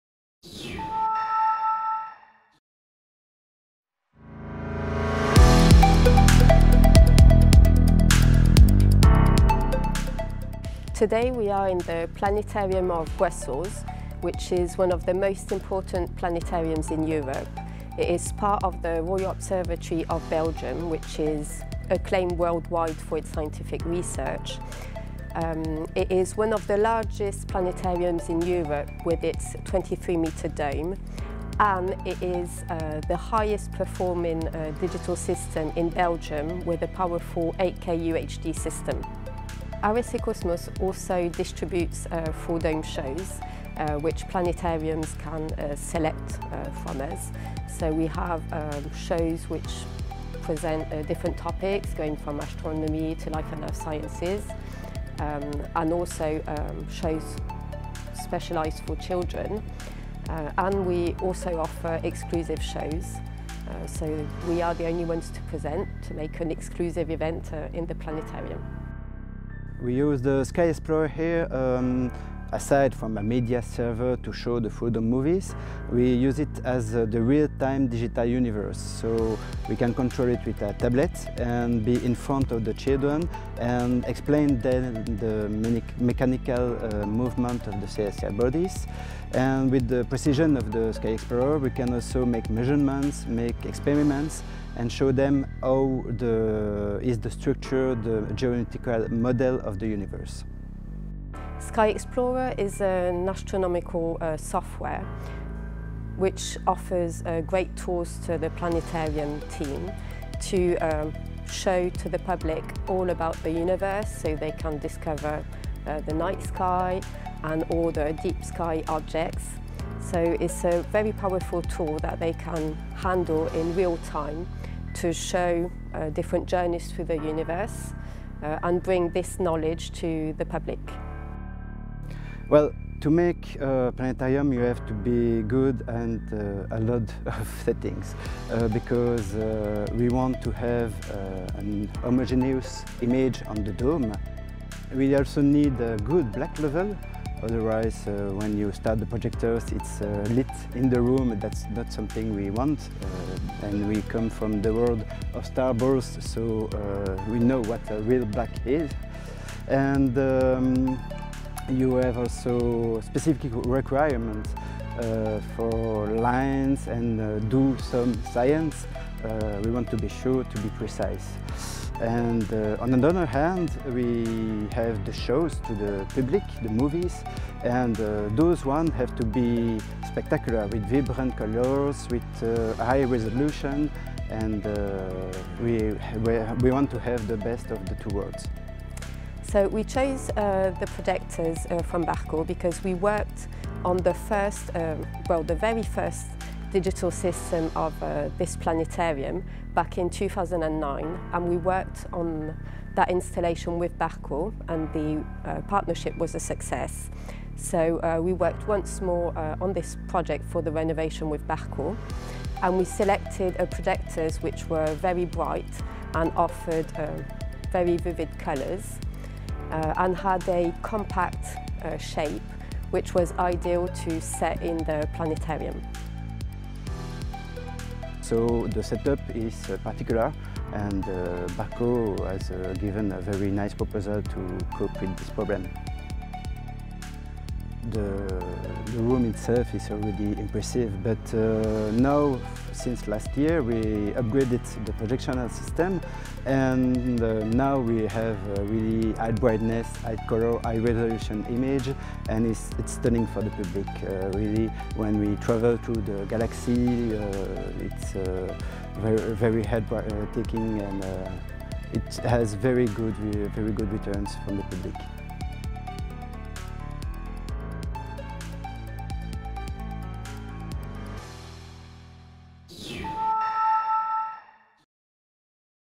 Brussels Planetarium offers stunning 360° view on the stars thanks to Barco and RSA Cosmos
- In April 2021 the Planetarium of Brussels opened its newly renovated dome, equipped with 8 state-of-the-art Barco projectors. The projectors were installed by RSA Cosmos, who also provided the latest version of its powerful astronomical software, SkyExplorer 2021.